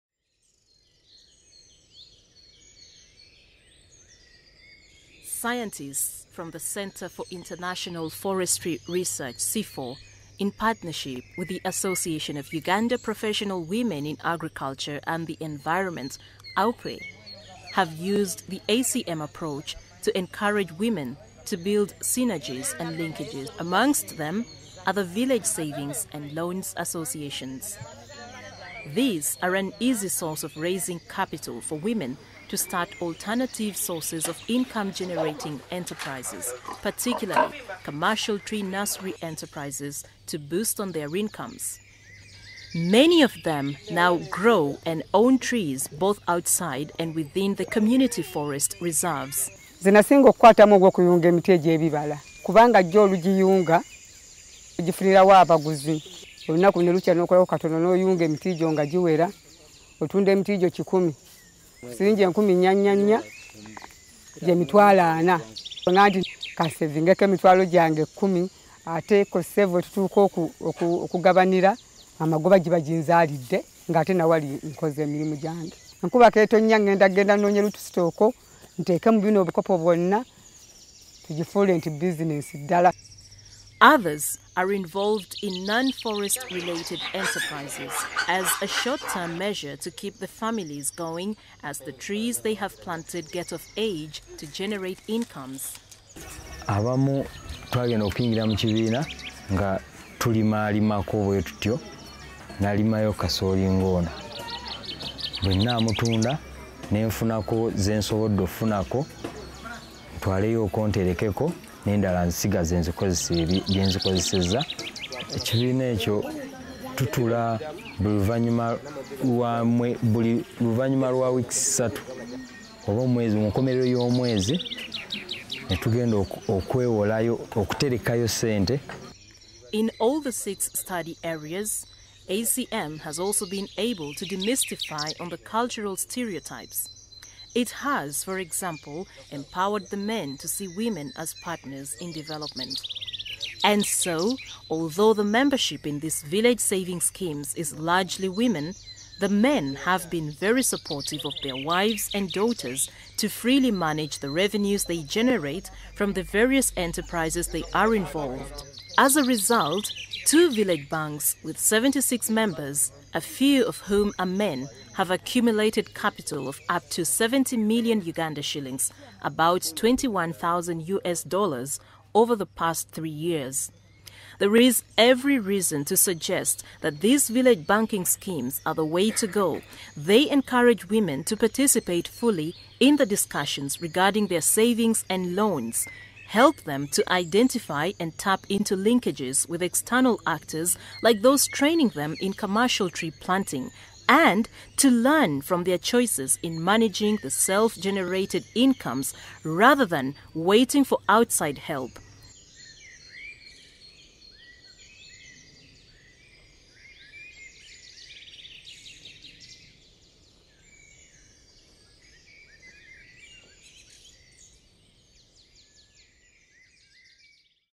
Forests and village saving schemes in Uganda
In Uganda, an Adaptive Collaborative Management project is promoting village saving schemes as a way to empower communities, build businesses and achieve self-generated income.